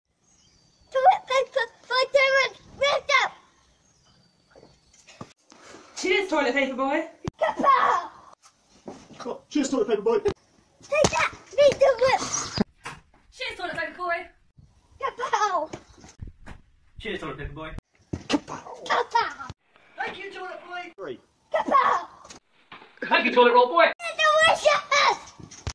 Toilet paper boy
In a world struck by coronavirus, people panic stockpilers strip the shelves, no toilet paper anywhere, luckily one superhero can save the day. Introducing toilet paper boy!!